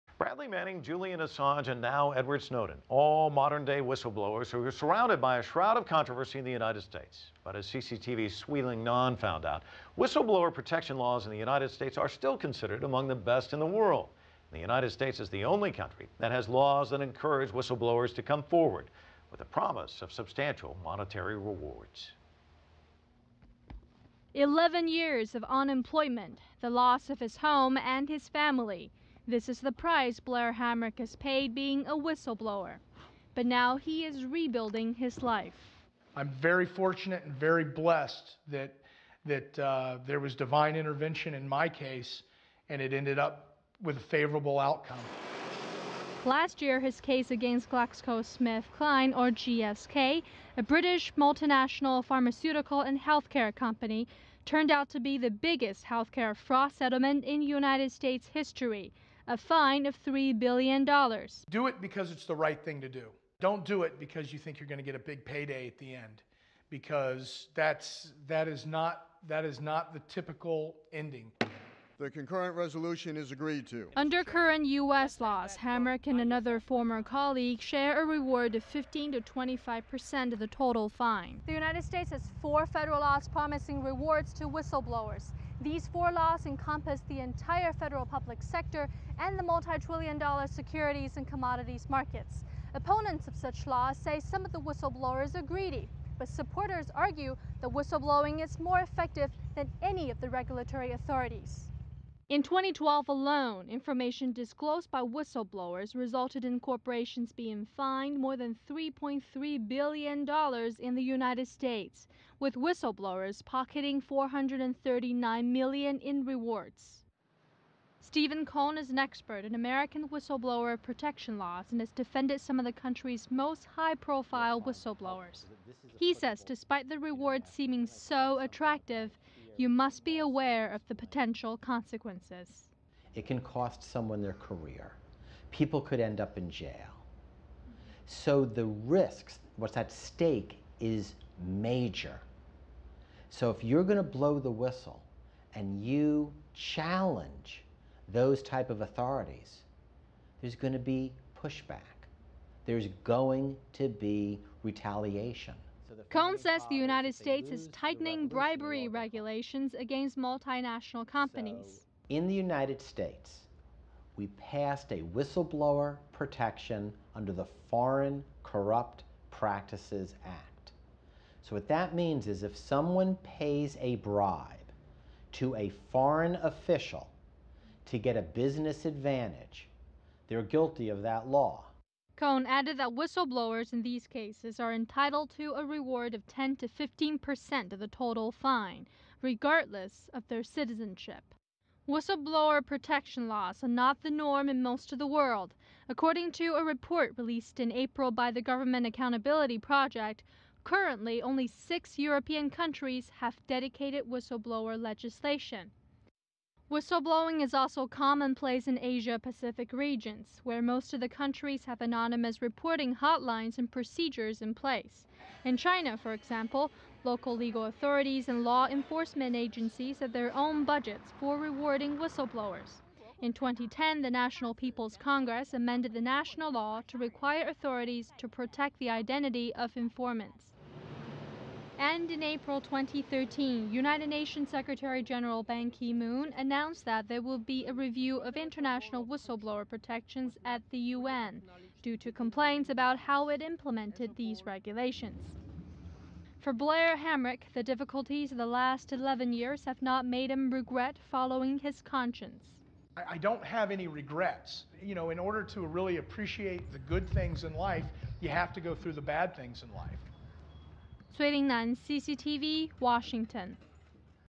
U.S. Remains Whistleblowing Haven despite Contradicting High Profile Cases
Correspondent Cui Lingnan takes an in-depth look at the large amount of protection whistle blowers receive in the United States.